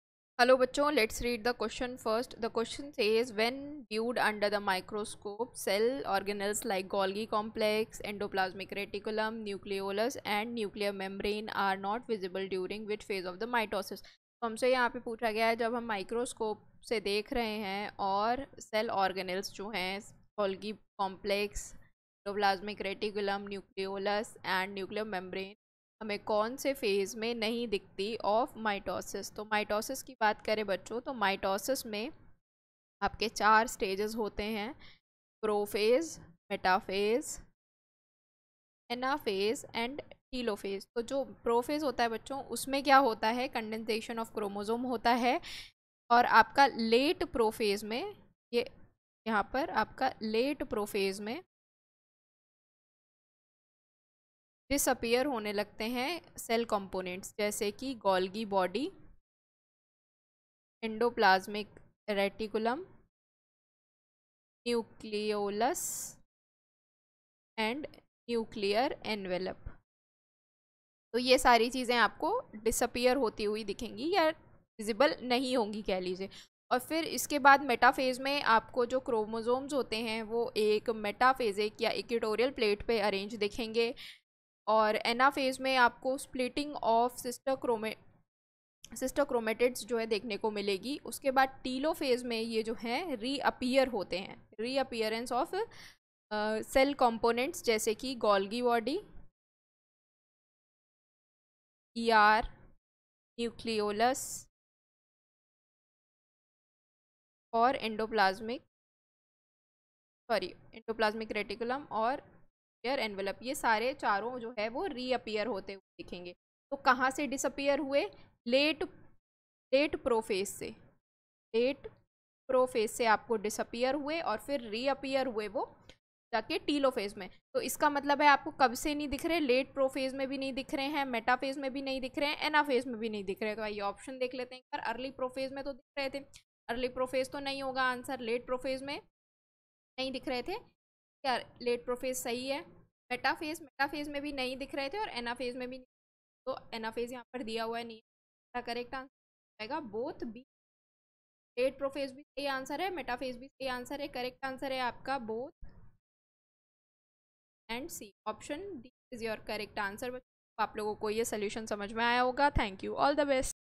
When viewed under the microscope, cell-organelles like Golgi complexes, endoplasmic reticulum, n....
When viewed under the microscope, cell-organelles like Golgi complexes, endoplasmic reticulum, nucleolus and nuclear envelope are not visible during which phase of mitosis?